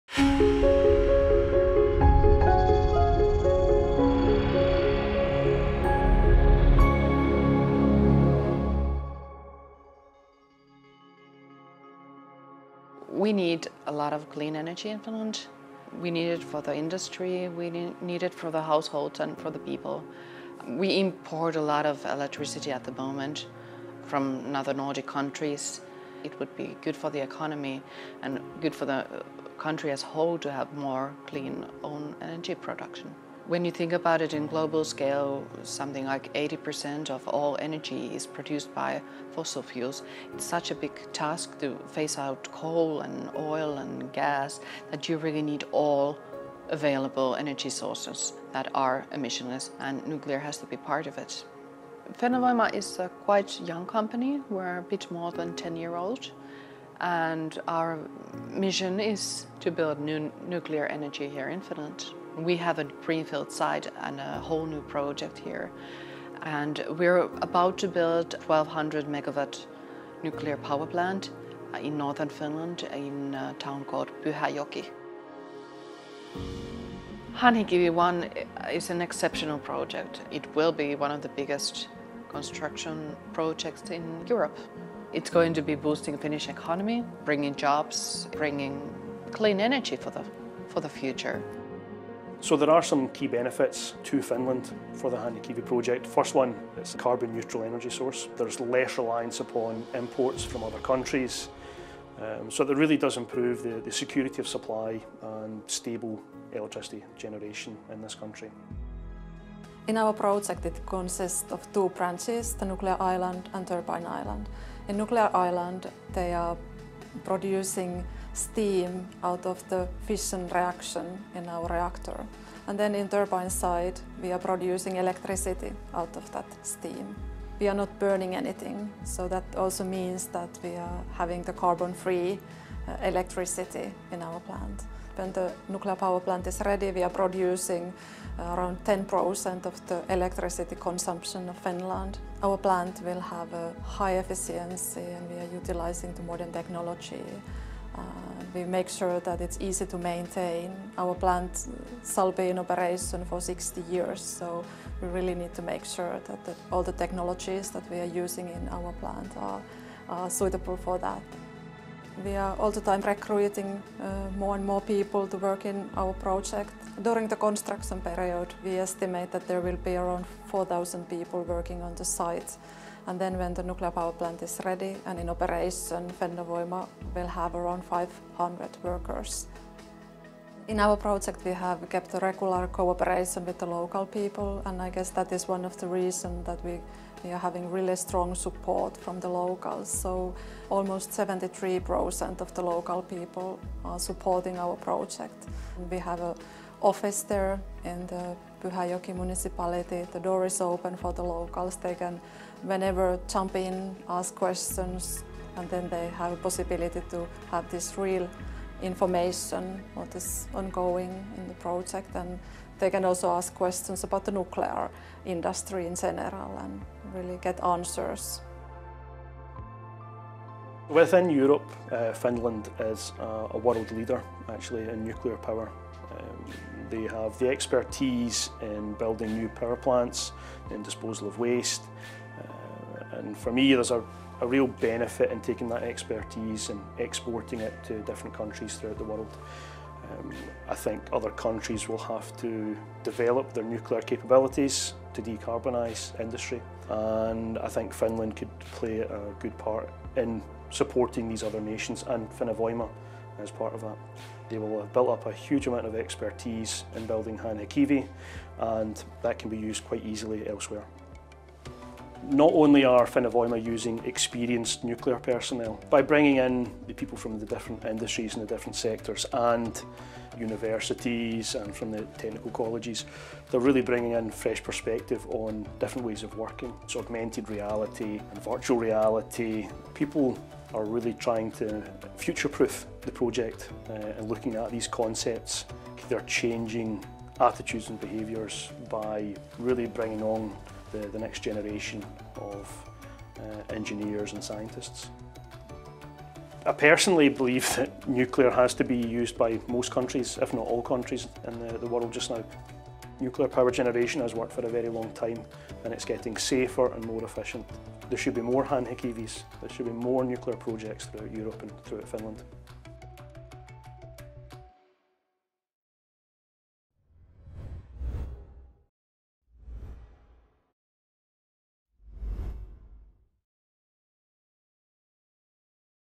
Fennovoima: Finland needs clean energy
In this video, Fennovoima’s experts are talking about the role of nuclear energy in off setting climate change. Fennovoima’s mission is to build Hanhikivi 1, a 1200 MW nuclear power plant in Northern Finland, to provide stable carbon-free energy.

During the construction phase, approximately 4,000 employees will be working at the site. Operational plant will employ about 500 people. Hanhikivi 1 has a strong local support – almost 73% of the Pyhäjoki residents are in favour of the project.

In Europe Finland is a leader in nuclear power. Other countries can use its expertise in NPPs operation and disposal of nuclear waste to decarbonize their energy.